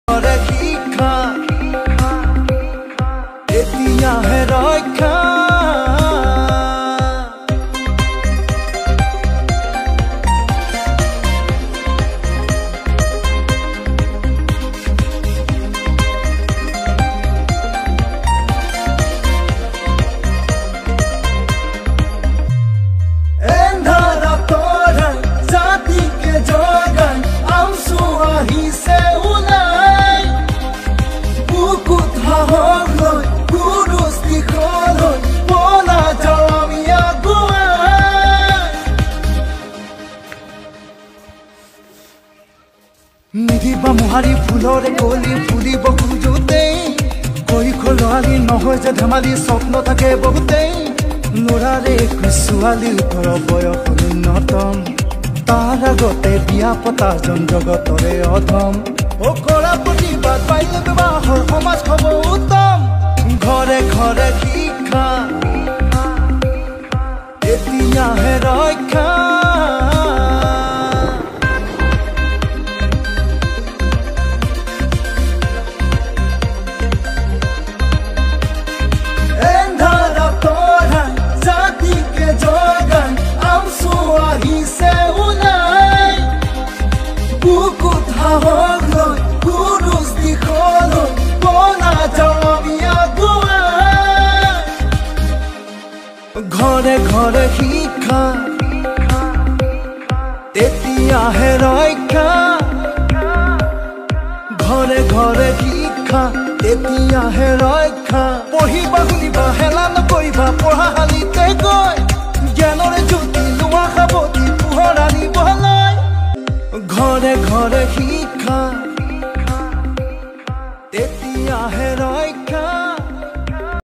First Division HSLC 2022 students Fakiragram | Mehdi Hassan
সদ্য ঘোষিত HSLCৰ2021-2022বৰ্ষৰ পৰীক্ষাৰ্থী লৈ ফকিৰাগ্রাম_আঞ্চলিক_সমিতি (আমছু)ৰ তৰফৰ পৰা সকলৈ আন্তৰিক শুভেচ্ছা ও হিয়াভৰা অভিনন্দন জ্ঞাপন জনালোঁ।। লগতে ফকিৰাগ্রামআঞ্চলিকৰ অন্তৰ্গত প্ৰথম বিভাগত উত্তীৰ্ণ হোৱা ছা্ত্ৰ_ছাত্ৰীক_সম্বোধনা_জনোৱাৰ লগতে তেওঁলোকৰ উজ্বল ভৱিষ্যত কামনা কৰিলোঁ৷।  তাৰে কেইটা মান আলোকচিত্ৰ

Fakiragram AAMSU feliciation program of HSLC  2022 first division holder students.

Motto :
        ঘৰে_ঘৰে_শিক্ষা,
        তেতিয়াহে_ৰক্ষা।
       ঘৰে_ঘৰে_সম্বোধনা৷
       ফকিৰাগ্রাম_আঞ্চলিক_আমছু।

Credits
Fakiragram Ancholik AAMSU Ancholik Commettee.